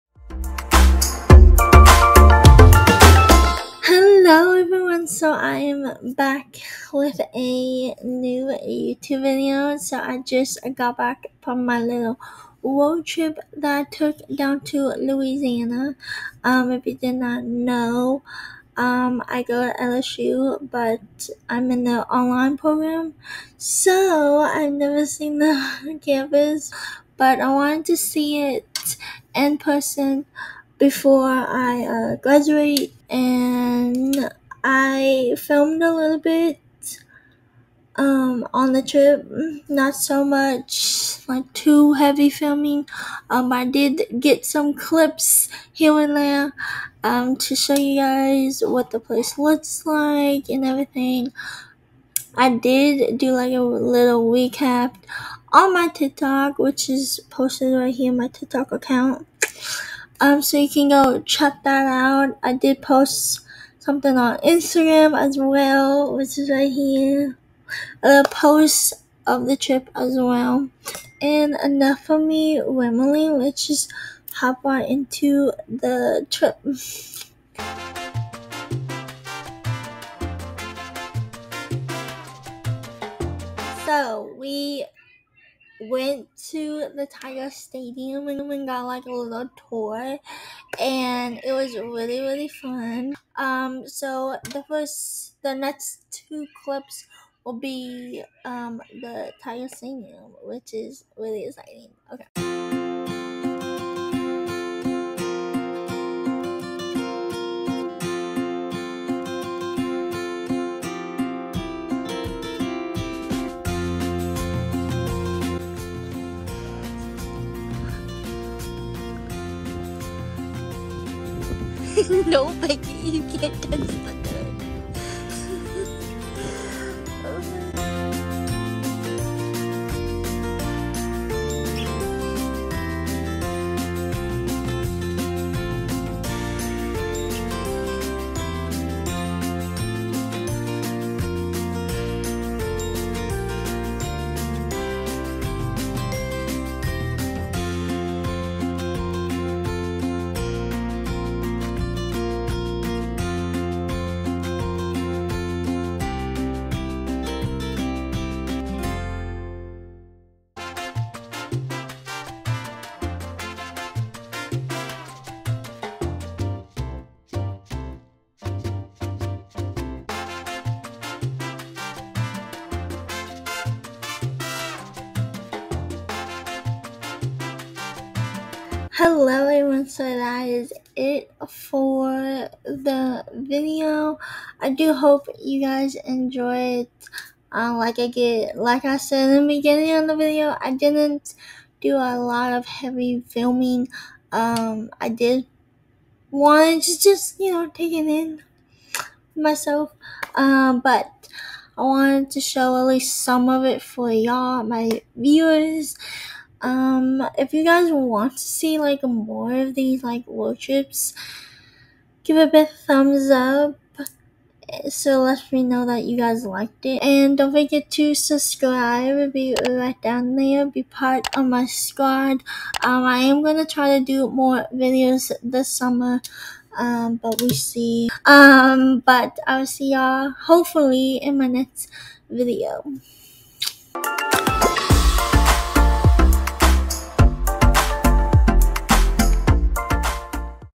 Road Trip to LSU 2k24
The first road trip of 2024. My family and I went to see my campus this year and it was a beautiful experience.